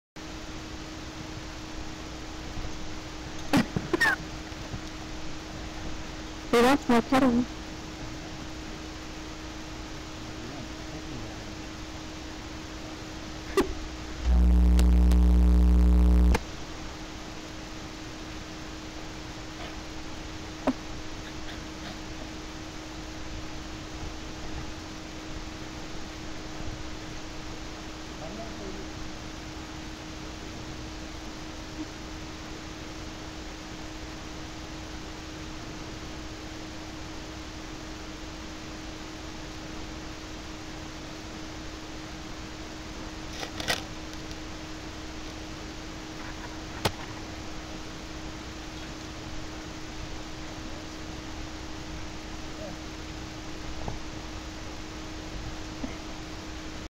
HAPPY POODLE
Toby the miniature poodle so cute and happy !